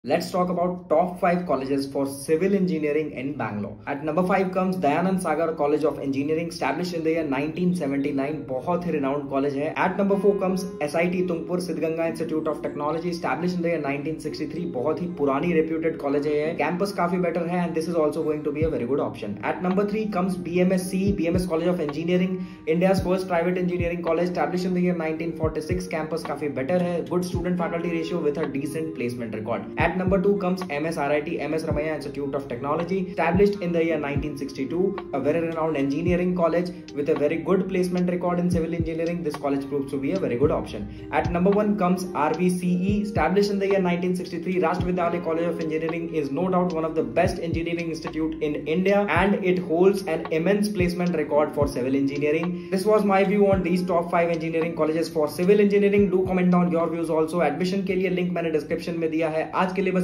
Top 5 Colleges For Civil Engineering in Bangalore🔥|| comedk2023 rvce #msrit
This video informs you about top 5 civil engineering colleges in Bangalore.